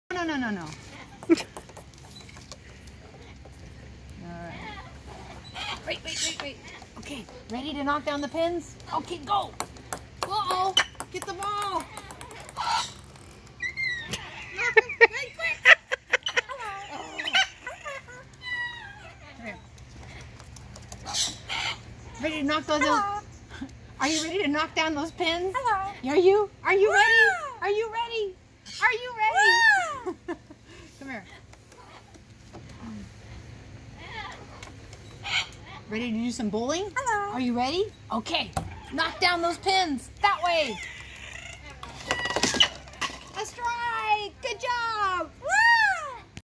Bowling Parrot 🎳
Piper the Slender Billed Cockatoo Bowling! 🎳